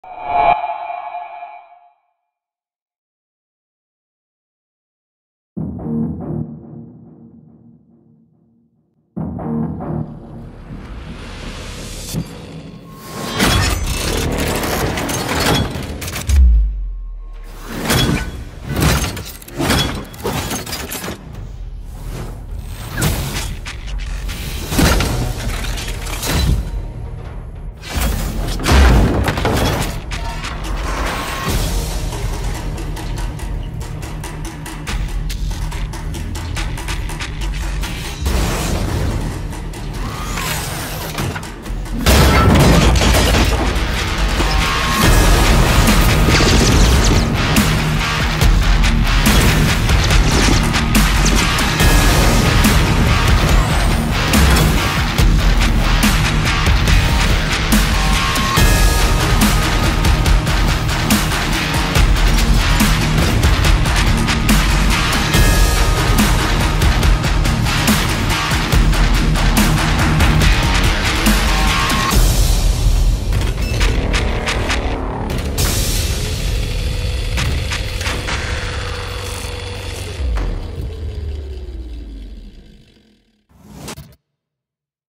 CGI & VFX Showreels: "Iron Man3 Sound Design & SFXl" - by Jaroslav Beck
Check out this awesome showreel demonstrating a completely recreated Sound Design, SFX for Iron Man 3 by the talented  Jaroslav Beck! He also composed the new music.

VFX by Pavel Kacerle
Sound Effects & Sound Design by Sqeepo